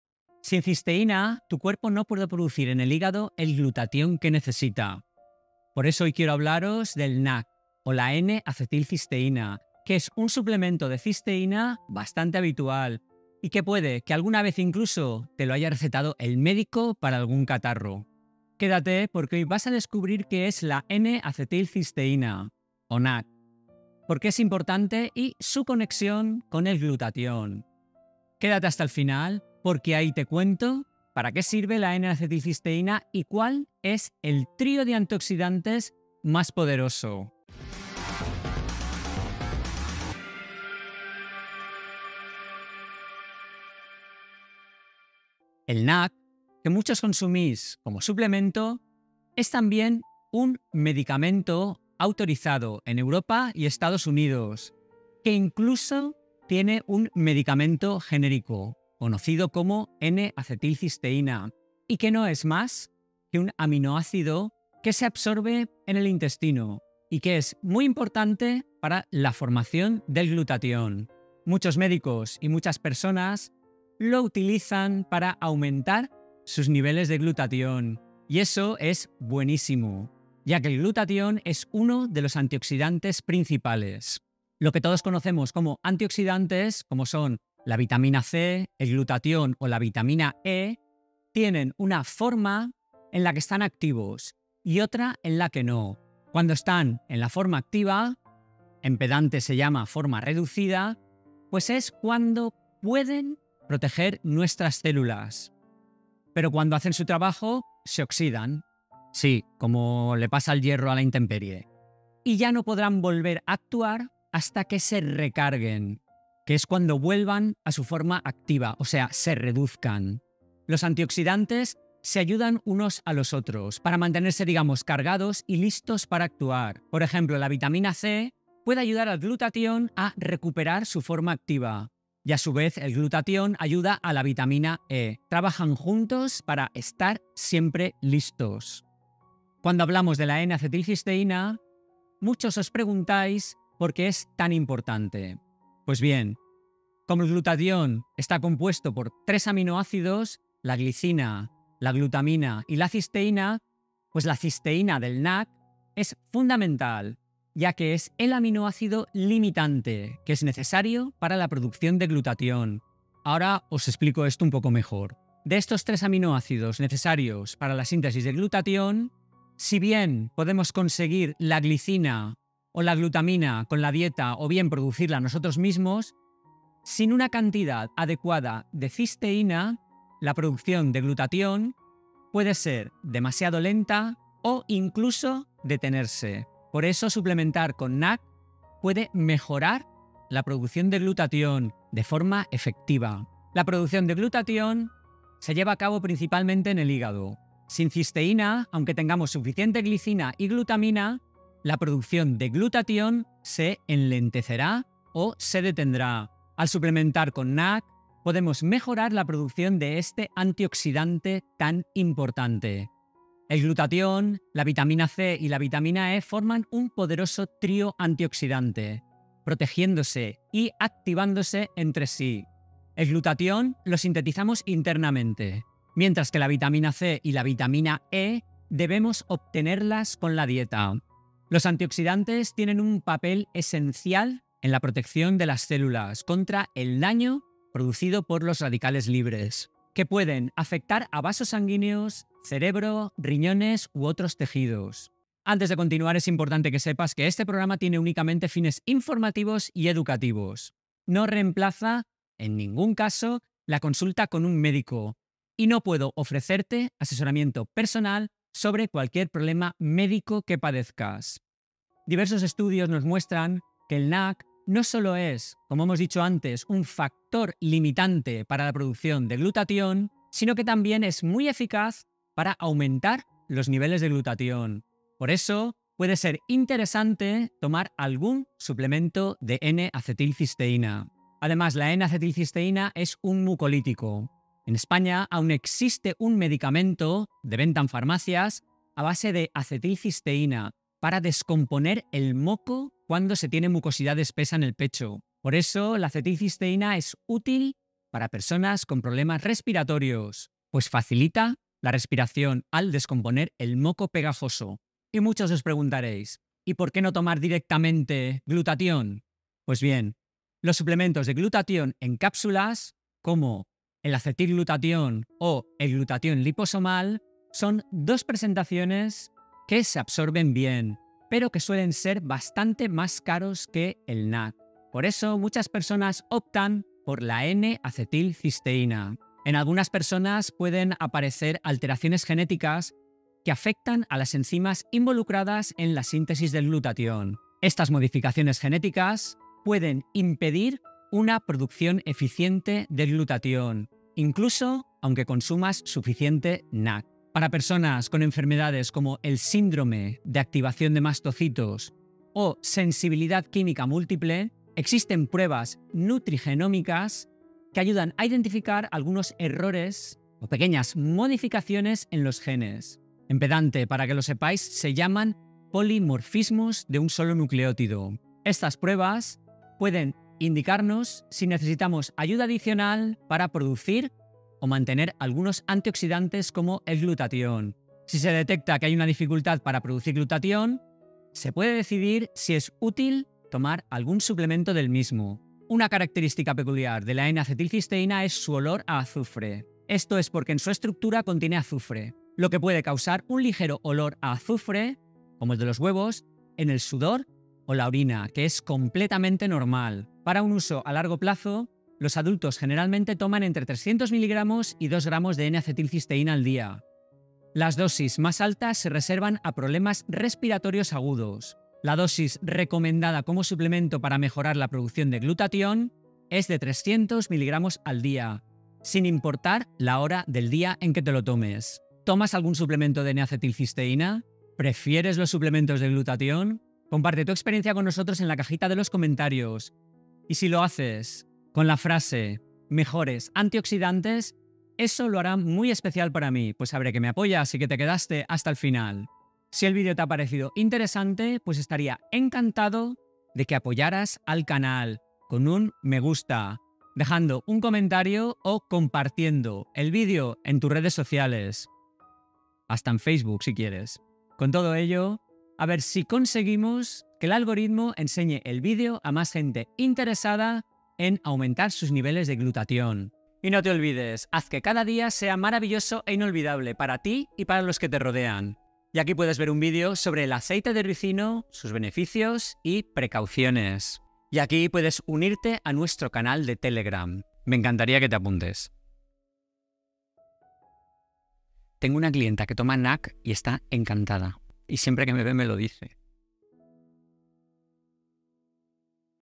Las VERDADERAS Razones para Usar N-Acetilcisteína NAC
Descubre las verdaderas razones para tomar N-acetilcisteína (NAC) y sus beneficios, cómo potencia el glutatión en tu cuerpo y mejora tu salud respiratoria. Aprende más sobre este antioxidante esencial y su papel en la defensa celular.

00:00 Introducción: Las VERDADERAS Razones para Usar N-Acetilcisteína (NAC)
00:45¿Qué es la n-acetilcisteína o NAC?
02:18 ¿Por qué es importante la N-Acetilcisteína? (Conexión con el glutatión)
03:32 El Trío Antioxidante (Vitamina C, Vitamina E, Glutatión)
04:21 ¿Para qué sirve NAC?
05:04 N-Acetilcisteina vs Glutatión
05:29 Incapacidad Genética para Producir Glutatión
06:25 Olor a Azufre de NAC
06:40 Dosificación del NAC y cómo tomarlo

¿Sabías que el NAC o N-acetilcisteína es clave para mejorar tus niveles de glutatión y apoyar tu sistema respiratorio? En este video te explico qué es el NAC, por qué es importante para el cuerpo y cómo ayuda a producir glutatión, el antioxidante más poderoso que tu hígado produce. La N-acetilcisteína es un suplemento ampliamente utilizado para quienes buscan un refuerzo antioxidante y para aquellos con problemas respiratorios, gracias a su capacidad para reducir la mucosidad.

Además, muchos médicos también lo recomiendan en ciertos casos para fortalecer el sistema de defensas del organismo.

Aprenderás:

• Qué es el NAC y cómo se convierte en cisteína para la síntesis de glutatión.
• Por qué el glutatión es fundamental para la protección celular y la salud del hígado.
• Cómo funciona el trío antioxidante: glutatión, vitamina C y vitamina E, y su poder conjunto.
• Diferencias entre tomar NAC y suplementos de glutatión.
• Dosis recomendadas de NAC y su uso como apoyo para problemas respiratorios.

Quédate hasta el final, porque te contaré las dosis recomendadas y el famoso "olor a azufre" del NAC, y cómo puedes aprovechar al máximo este suplemento para la salud general y el bienestar.

Bibliografía

1. Lu SC. Glutathione synthesis. Biochim Biophys Acta [Internet]. 2013;1830(5):3143–53.

2. Rushworth GF, Megson IL. Existing and potential therapeutic uses for N-acetylcysteine: the need for conversion to intracellular glutathione for antioxidant benefits. Pharmacol Ther [Internet]. 2014;141(2):150–9.

3. Kelly GS. Clinical applications of N-acetylcysteine. Altern Med Rev. 1998;3(2):114–27.

Información legal.
Vídeo de grabación propia.